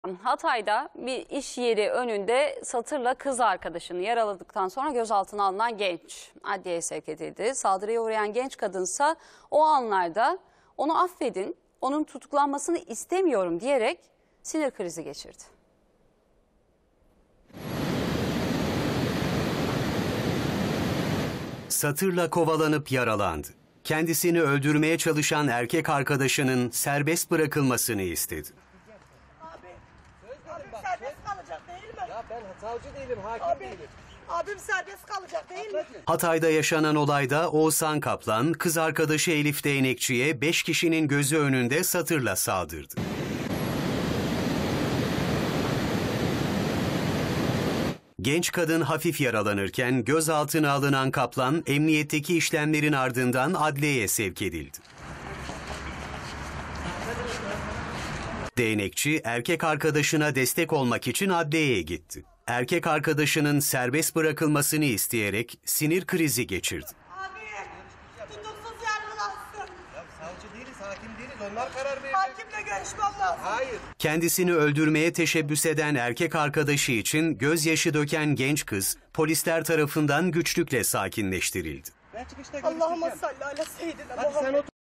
Türkiye'nin konuştuğu olayda akılalmaz gelişme! Kendisine satırla saldıran kişi için gözyaşı döktü!
Hatay'da tavla oynayan 5 kişinin gözü önünde, kız arkadaşı Elif D.'yi (17) satırla darbettiği suçlamasıyla gözaltına alınan Oğuzhan K. (24) adliyeye sevk edildi. Erkek arkadaşından şikayetçi olmayan Elif D.'yi, sevk sırasında Oğuzhan K.'ye sarılmak isterken polis engelledi. Oğuzhan K.'nin tutuksuz yargılanması için hakimle görüşmek istediğini söyleyen Elif D., adliyeye alınmayınca kendini yerden yere attı.

Olay, 8 Ekim günü saat 16.00 sıralarında, Antakya ilçesinde meydana geldi. Kendisini satırla kovalayan erkek arkadaşı Oğuzhan K.'den kaçan Elif D., bir dükkanın önünde tavla oynayan 5 kişinin yanına sığınıp yardım istedi. Bu sırada sesleri duymalarına rağmen yerinden kalkmayan, oyuna devam eden kişiler, satırlı saldırgan karşısında oturmaya devam etti. Oğuzhan K., 5 kişinin gözü önünde genç kıza defalarca vurdu. Bu anlar, iş yerinin güvenlik kamerasınca kaydedilip, sosyal medyadan paylaşıldı.
Hatay Valiliği, tepki çeken görüntülerle ilgili açıklamada bulundu. Oğuzhan K.'nin yakalandığı, yaralı Elif D.'nin de hayati tehlikesinin olmadığı belirtilen açıklamada, "Sosyal medyada görüntüleri paylaşılan kadına şiddet olayı ile ilgili, Valiliğimizce yapılan araştırmalar neticesinde olayın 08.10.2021 saat 16.00 sıralarında Antakya ilçemizde gerçekleştiği, şüphelinin emniyet birimlerimizin çalışmaları sonucunda yakalandığı, şiddete maruz kalan vatandaşımızın hayati tehlikesi olmadığı anlaşılmıştır. Kadına yapılan her türlü şiddetin karşısında olmakla beraber, kadına verilen değerin, her türlü fayda ve beklentinin ötesinde, insan haklarının bir gereği olarak bilir, kadına yönelik şiddet tedbirlerini sıfır tolerans ilkesiyle hayata etkin şekilde geçirmek için tüm birimlerimizce çalışmaya devam ediyoruz" denildi.

ŞİKAYETÇİ OLMADI

Bölgede 'Kurbat' olarak bilinen aşiretin mensubu olduğu belirtilen Elif D. ile Oğuzhan K.'nin, aralarındaki anlaşmazlık nedeniyle sokak ortasında kavga ettiği öğrenildi. Elif D., güvenlik kamerasına yansıyan olaydan sonra gözaltına alınan Oğuzhan K.'den şikayetçi olmadı.

ADLİYEDE HAKİMLE GÖRÜŞMEK İSTEDİ

Emniyetteki işlemleri tamamlanan Oğuzhan K., bugün adliyeye sevk edildi. Satırlı saldırıya uğrayan Elif D. de adliyeye gelerek erkek arkadaşını yalnız bırakmadı. Polis aracından indirilip adliyeye götürülen Oğuzhan K.'ye sarılmak isteyen Elif D.'yi polis engelledi. Daha sonra adliyeye girmek isteyince de engellenen Elif D., kendini yere attı. Elif D., kendisini engelleyen polis memurlarına, "Kurban olayım lütfen. Hakimle görüşmem lazım. Tutuksuz yargılansın" dedi. Oğuzhan K.'nin adliyedeki işlemleri devam ediyor.